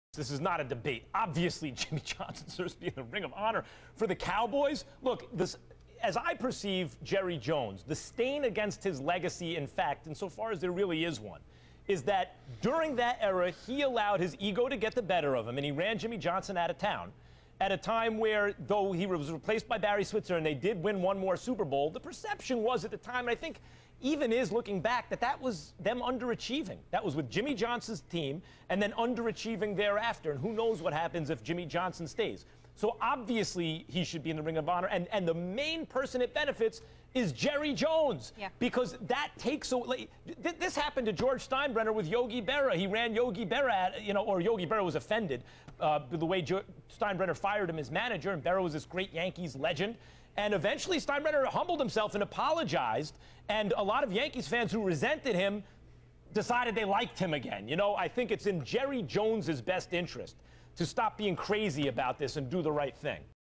Kellerman: Jerry Jones let ego get best of him
NFL
Kellerman: Jerry Jones let ego get best of him Max Kellerman says it's obvious Jimmy Johnson should be in the Cowboys Ring of Honor and the only thing keeping him out is Jerry Jones' ego.